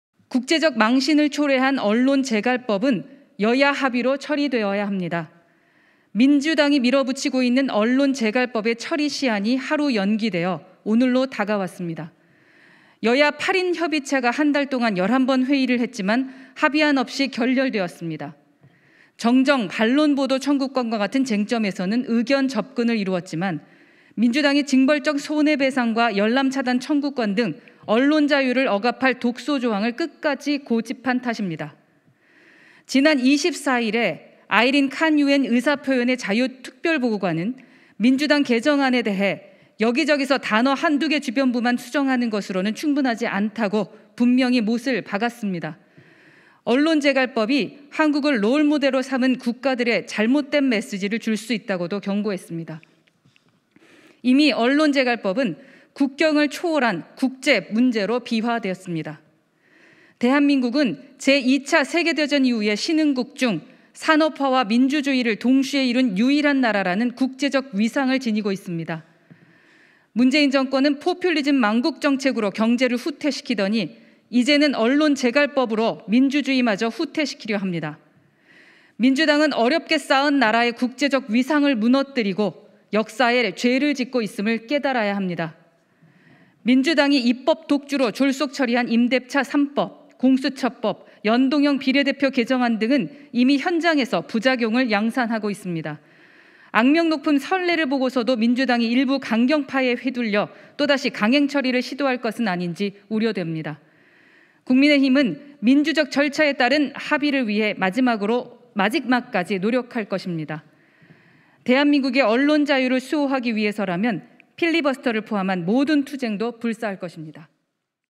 허은아 "국민의힘, 언론의자유 위해 필리버스터 포함 투쟁 불사"
허은아 국민의힘 의원이 28일 국회에서 소통관에서 열린 기자회견에서 언론중재법 관련해 발언하고있다.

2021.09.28 일요서울TV 김동현 기자